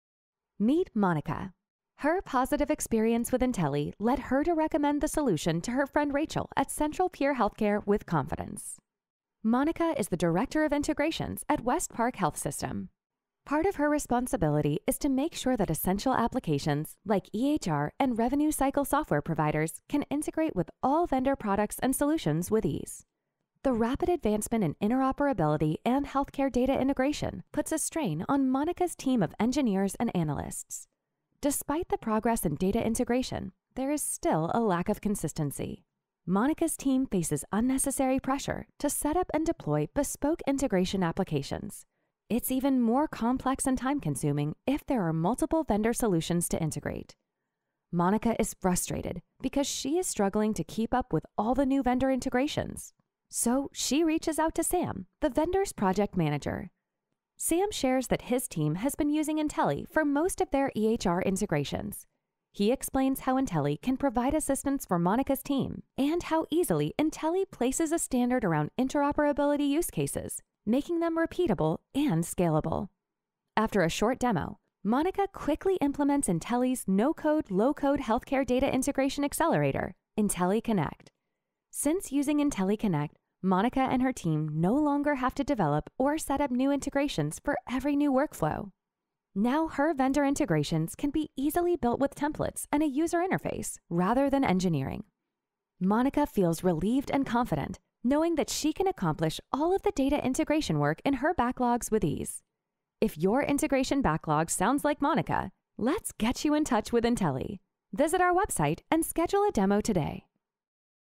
Director of Integration | EHR & Vendor Data Integration | intely
Meet Monica. Her positive experience with intely led her to recommend the solution to her friend Rachel at Central Pier Healthcare with confidence.

Monica is the Director of Integration at West Park Health System. Part of her responsibility is to make sure that essential applications, like EHR and Revenue Cycle software providers use, can integrate with all vendor products and solutions with ease.

The rapid advancement in interoperability and healthcare data integration puts a strain on Monica’s team of engineers and analysts.

Despite the progress in data integration, there is still a lack of consistency. Monica’s team faces unnecessary pressure to set up and deploy bespoke integration applications. It’s even more complex and time-consuming if there are multiple vendor solutions to integrate.

Monica is frustrated because she is struggling to keep up with all the new vendor integrations. So, she reaches out to Sam, the vendor’s project manager.

Sam shares that his team has been using intely for most of their EHR integrations. He explains how intely can provide assistance for Monica’s team and how easily intely places a standard around interoperability use cases, making them repeatable and scalable.

After a short demo, Monica quickly implements intely’s no code/low code healthcare data integration accelerator – intelyConnect.

Since using intelyConnect, Monica and her team no longer have to develop or set up new integrations for every new workflow. Now, her vendor integrations can easily be built with templates and a user interface rather than engineering.

Monica feels relieved and confident, knowing that she can accomplish all of the data integration work in her backlogs with ease.

If your integration backlog sounds like Monica’s, let’s get you in touch with Intely!